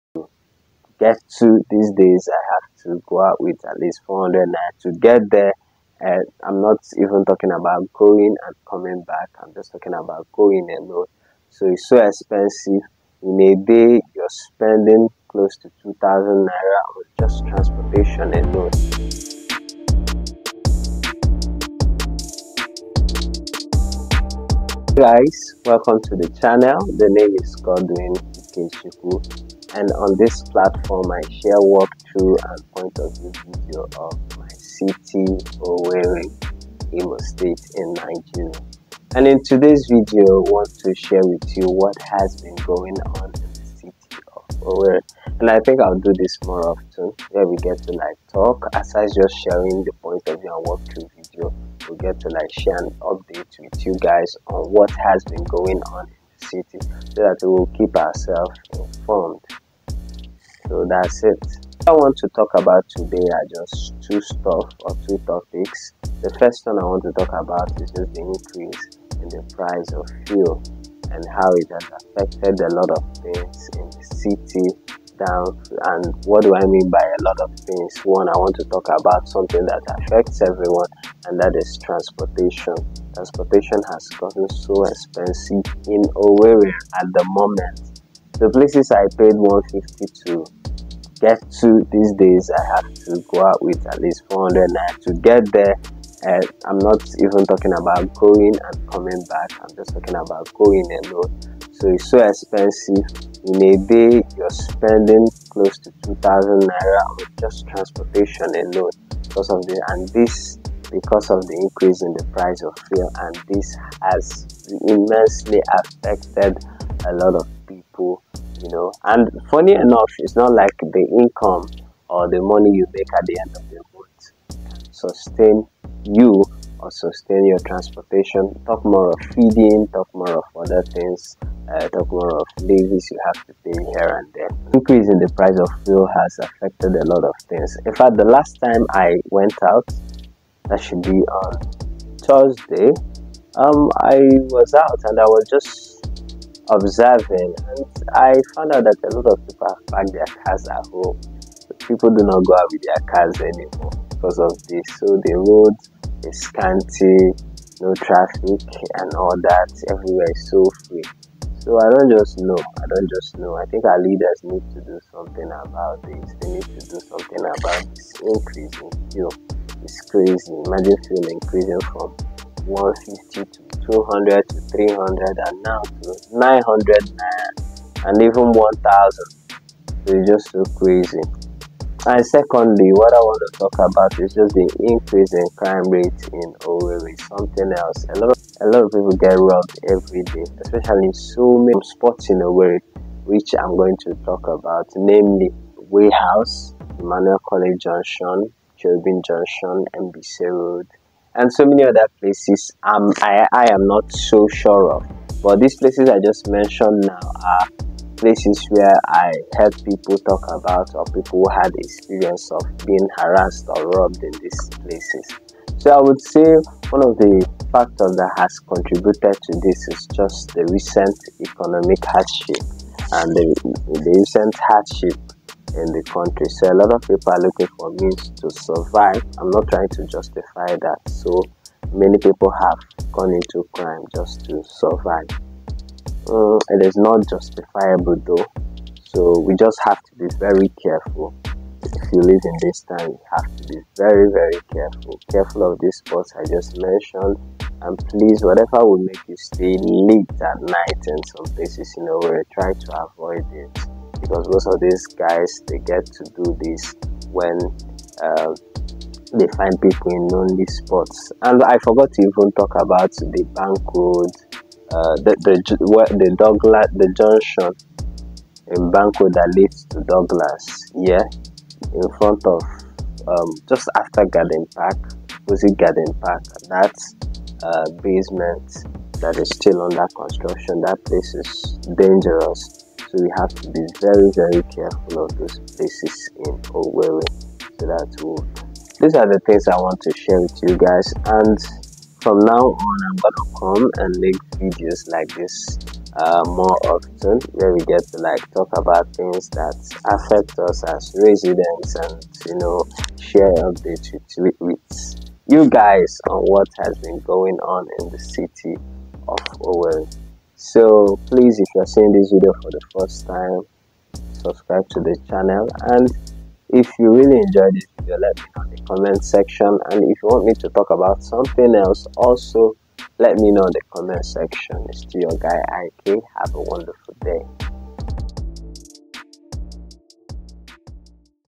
If you live in Owerri watch this |Fuel price |Cost of transportation |Crime
Get the latest update on Owerri's pressing issues! Join me as I discuss the alarming fuel price hike & its impact on transportation
Rising crime rates: armed robbery hotspots & safety concerns.